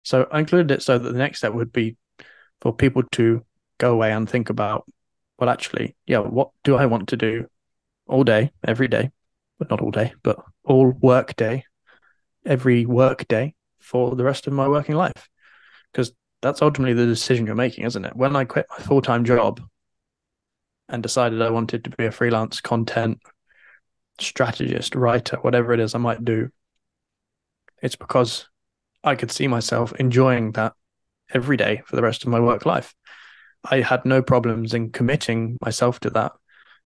When I Quit My Full Time Job | Dominic Kent Interview
Learn how to become a freelance writer and live a more autonomous life.

Dominic Kent is a freelance writer and author of the book "The Autonomous Freelancer".